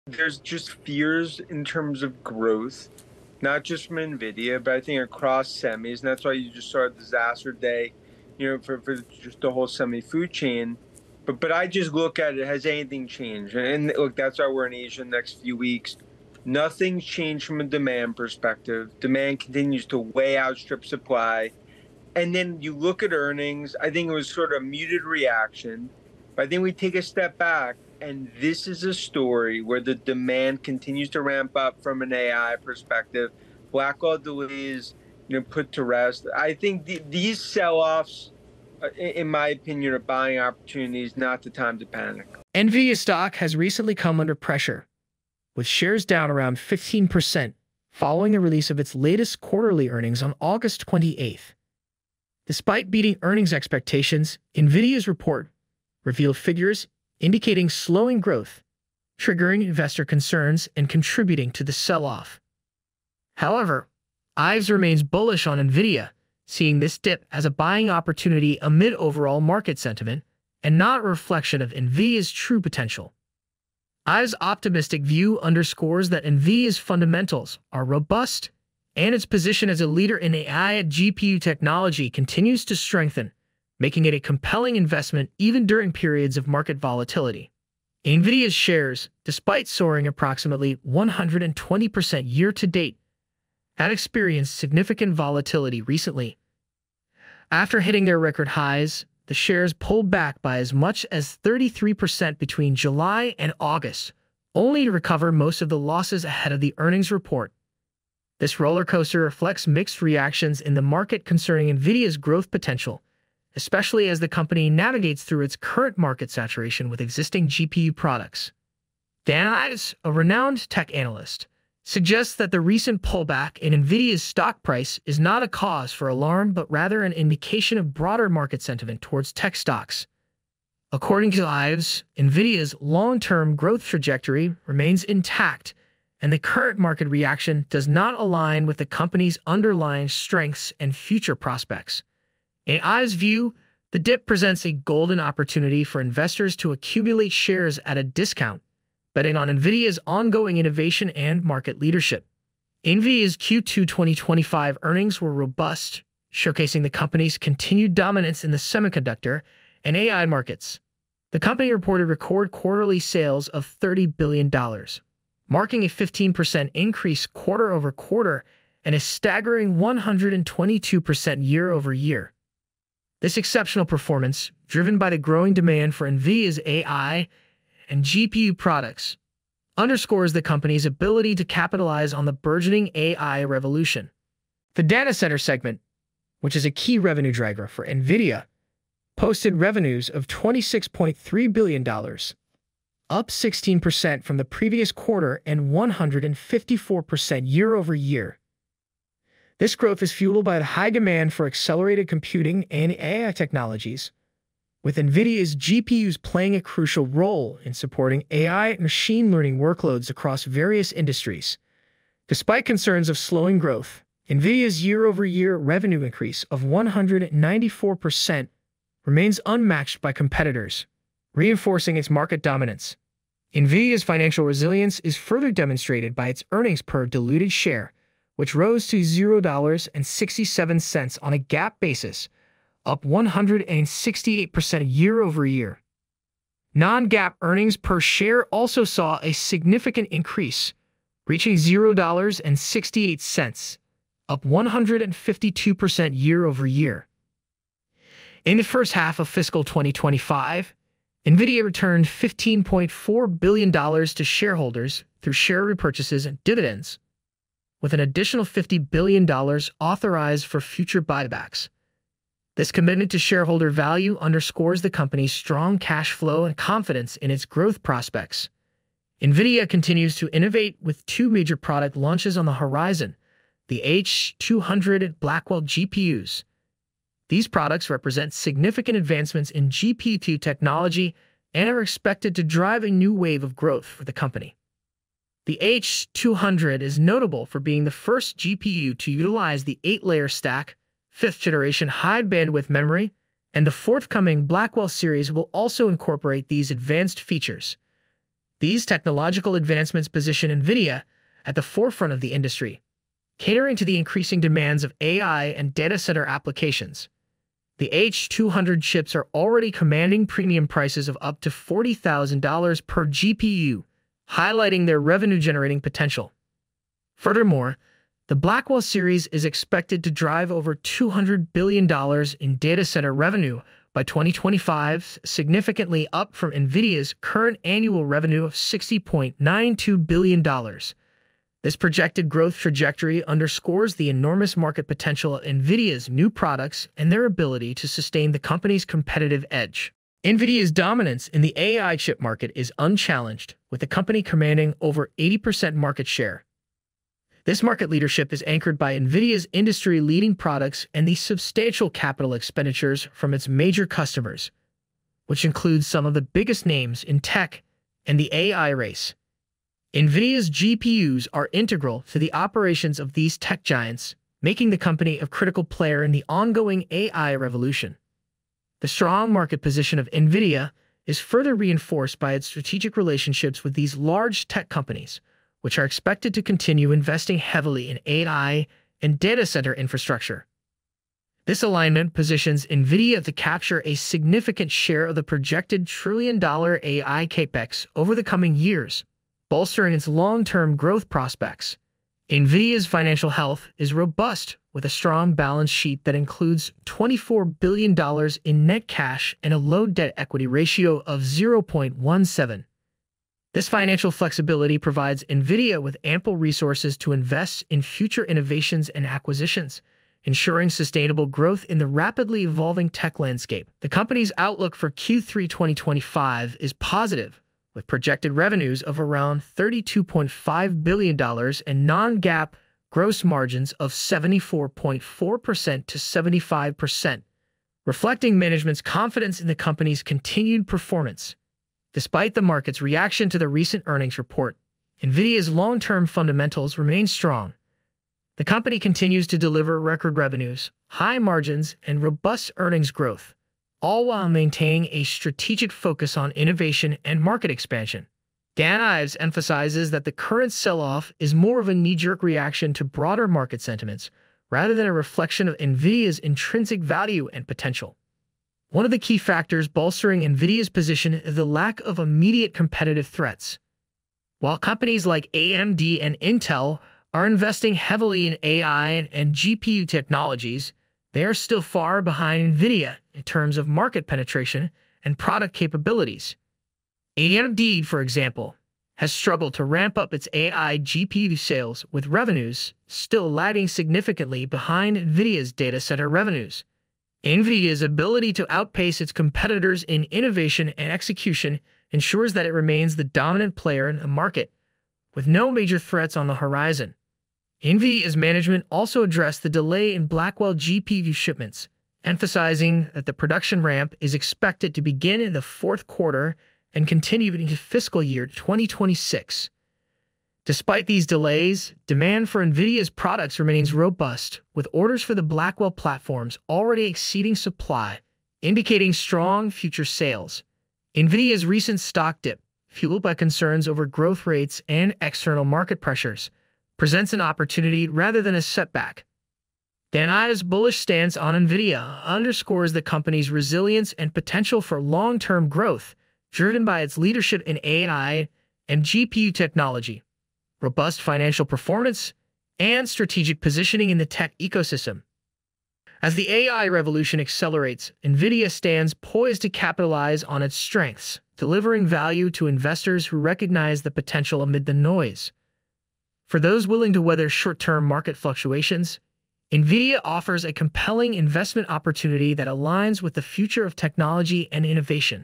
"This Dip Will Turn You Into A Millionaire" Dan Ives Says "Get Ready For Explosive Growth, Act Now!
Nvidia stock has recently come under pressure, with shares down around 15% following the release of its latest quarterly earnings on August 28th. Despite beating earnings expectations, Nvidia's report revealed figures indicating slowing growth, triggering investor concerns and contributing to the sell-off. However, Ives remains bullish on Nvidia, seeing this dip as a buying opportunity amid overall market sentiment and not a reflection of Nvidia's true potential. Ives’ optimistic view underscores that Nvidia's fundamentals are robust, and its position as a leader in AI and GPU technology continues to strengthen, making it a compelling investment even during periods of market volatility.
Nvidia's shares, despite soaring approximately 120% year-to-date, have experienced significant volatility recently. After hitting their record highs, the shares pulled back by as much as 33% between July and August, only to recover most of the losses ahead of the earnings report. This rollercoaster reflects mixed reactions in the market concerning Nvidia’s growth potential, especially as the company navigates through its current market saturation with existing GPU products.